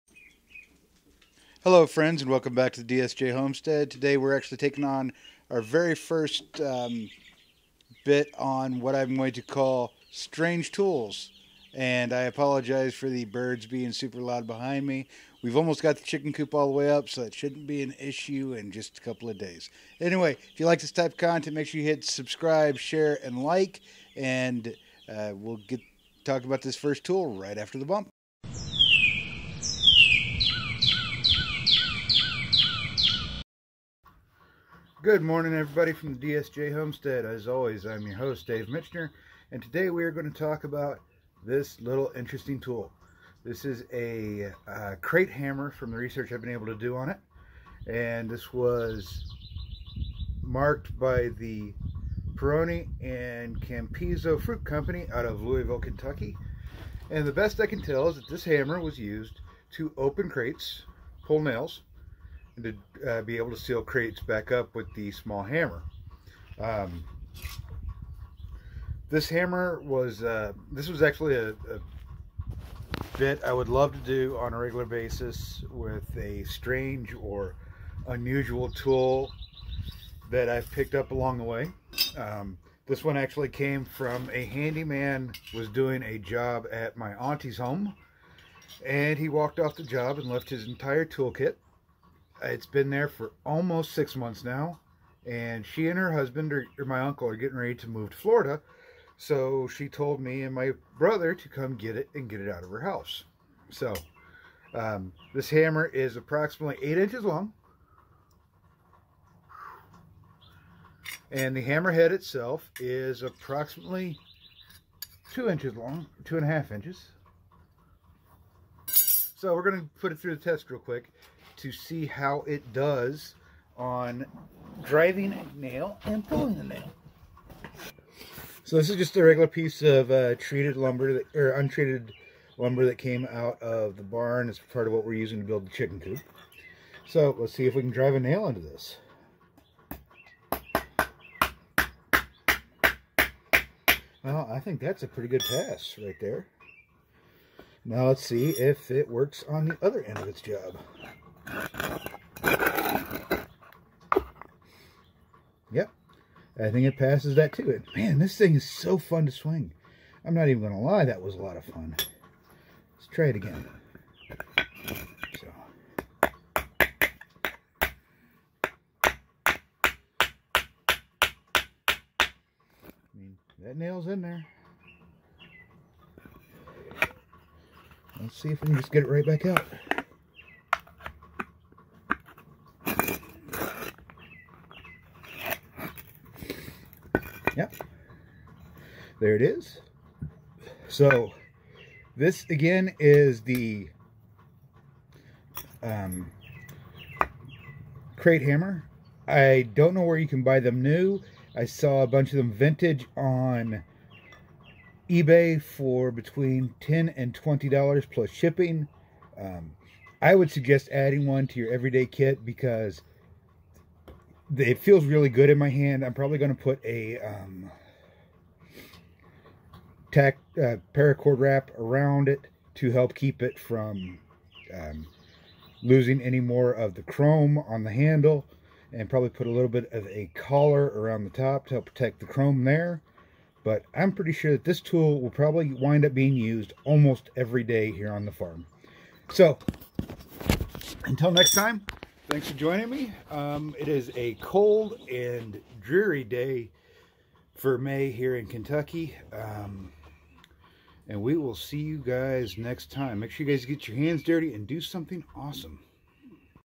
Strange Tools 1: The Crate Hammer
I found this while helping my aunt and uncle pack to move to Florida. Apparently, it was left behind by a handyman six to eight months ago. But I'd never seen this tool before, and I'm glad I found it. It'll probably be one of my most used tools!